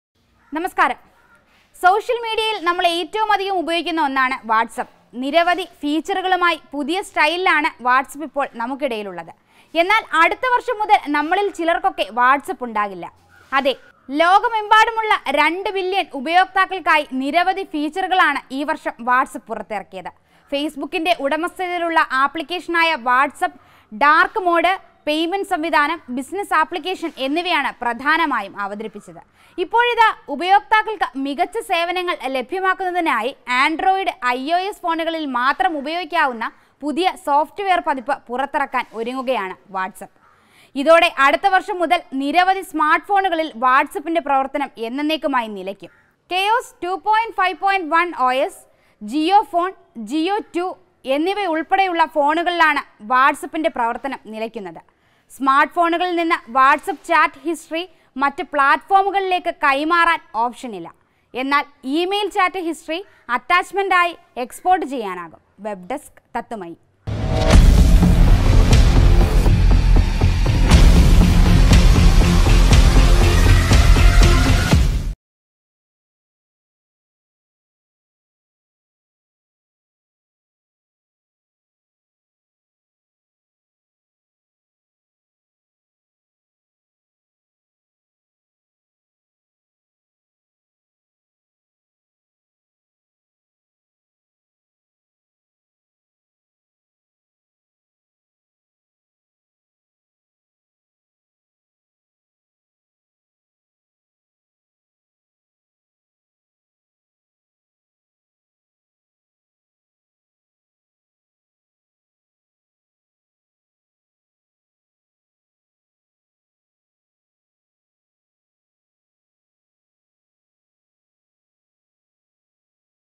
2021 മുതൽ whatsapp ഇല്ല.!! പകരമൊരു ആപ്ലിക്കേഷൻ ഉണ്ടാകുമോ? സമൂഹമാധ്യമങ്ങൾ ആശങ്കയിൽ
2021 മുതൽ whatsapp ഇല്ല.!! പകരമൊരു ആപ്ലിക്കേഷൻ ഉണ്ടാകുമോ? സമൂഹമാധ്യമങ്ങൾ ആശങ്കയിൽ | Whatsapp